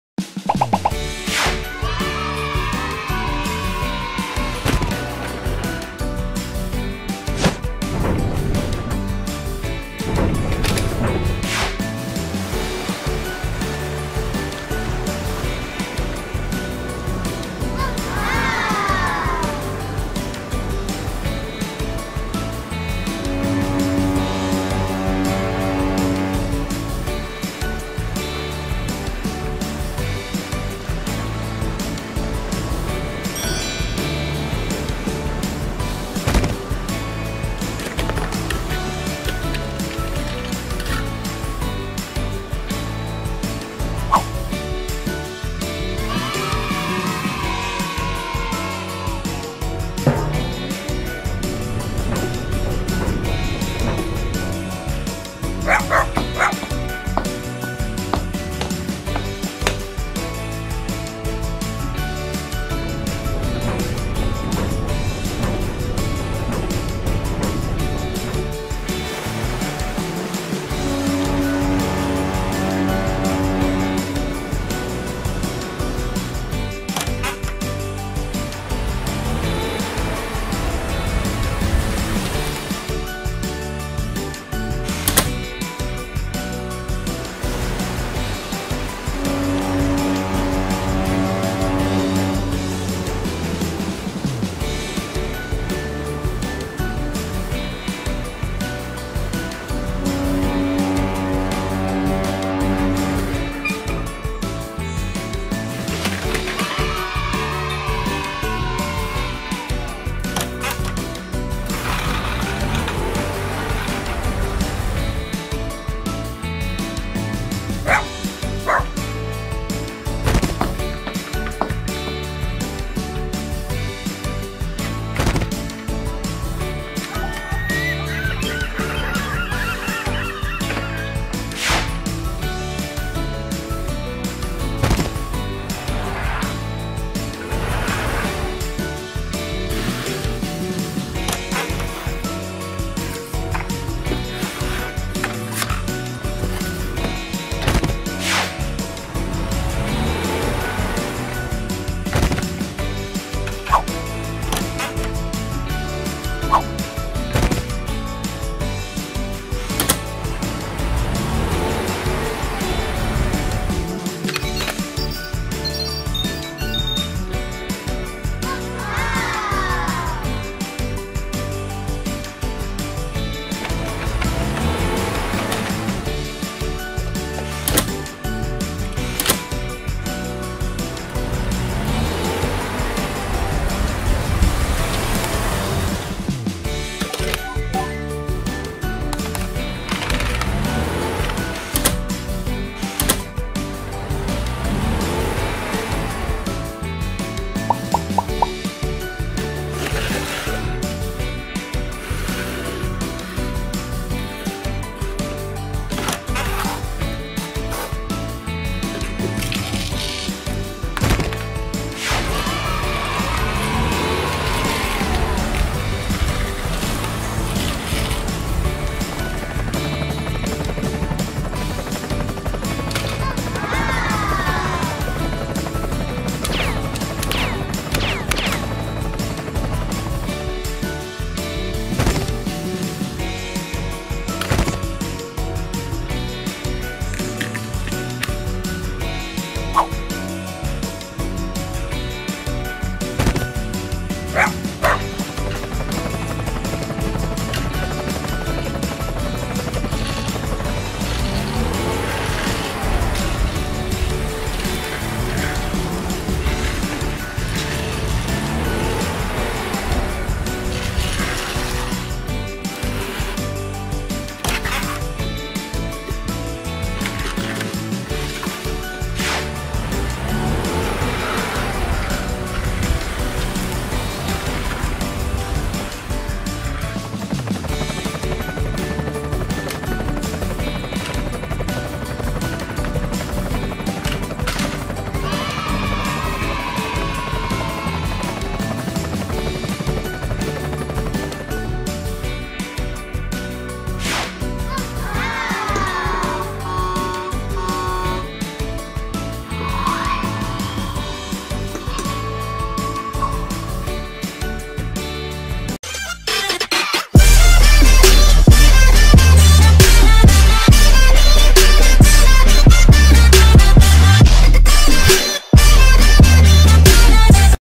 Paw Patrol: Ryder, Chase, Skye & Debris Toy Car | Toys for children
The little puppies from Paw Patrol are back! Today we show you many Paw Patrol toys: Paw Patroller, Flip and Fly and many more!